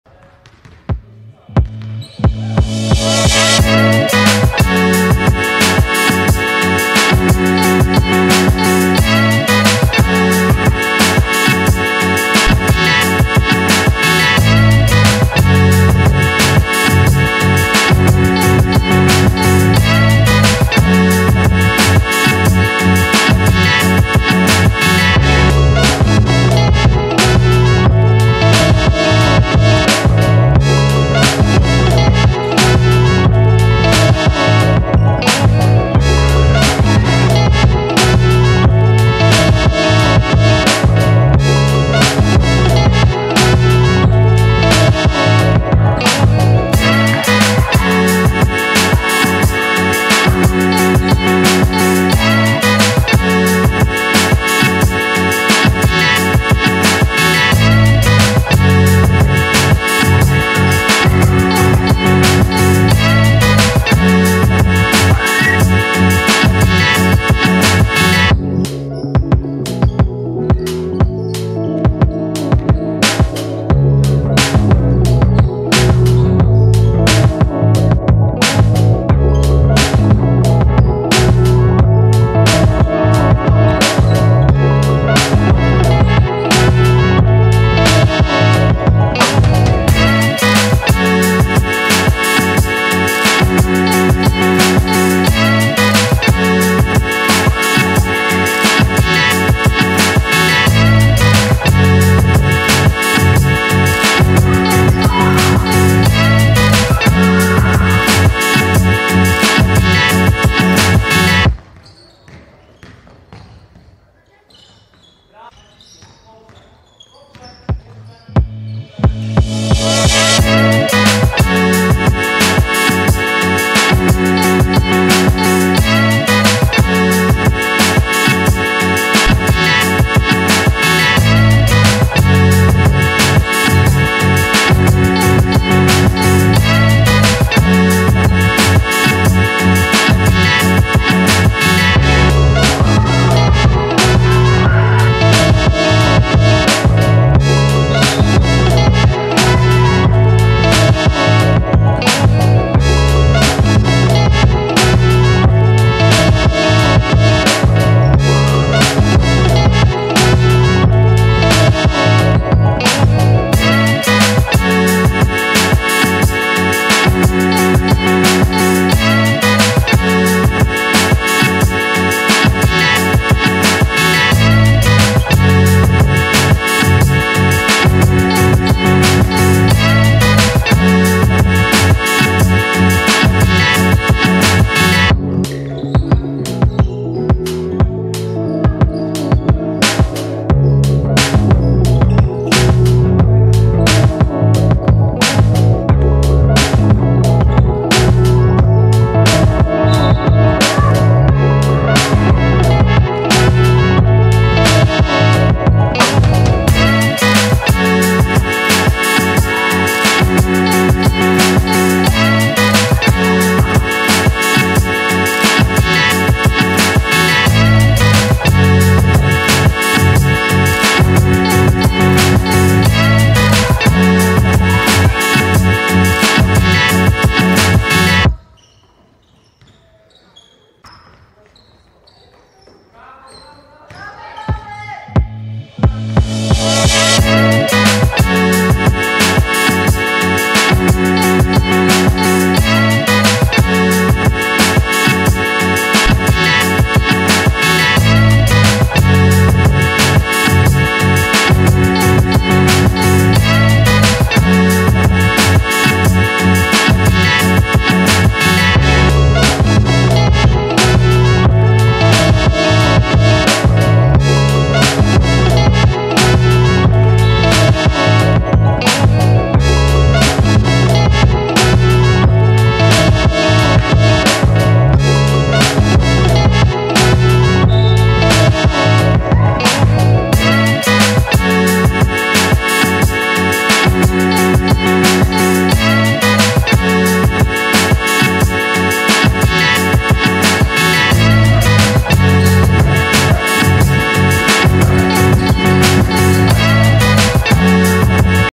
08 10 Sun vs Aces – Girls League Elite Jesień 2023
Sun 52:61 Aces
M.Niewiadomska 18pkt 9zb 6stl 3blk
H.Tynel 18pkt 21zb 4ast 8blk